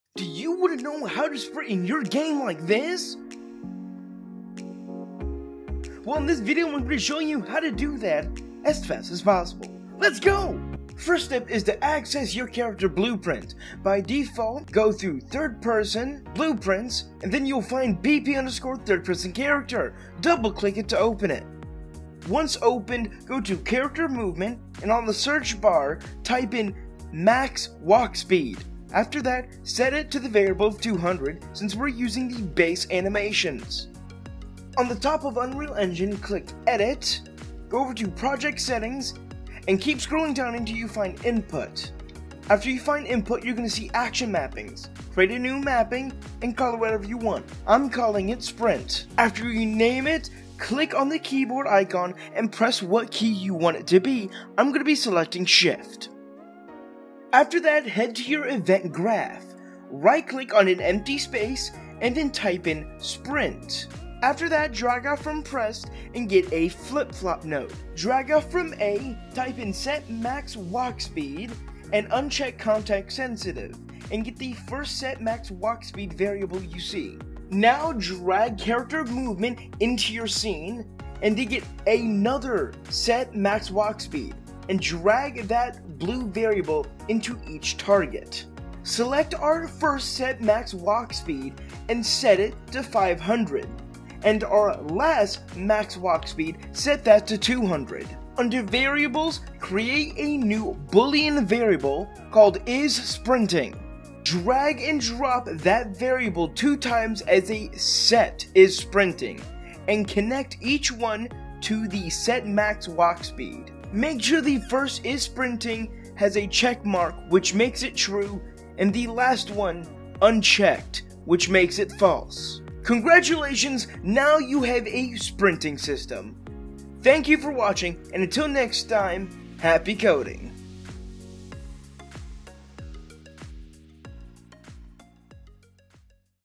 How To Make A Sprinting System In 2 Minutes! | UE4 & UE5
If You Would Like More Videos About UE Let Me Know.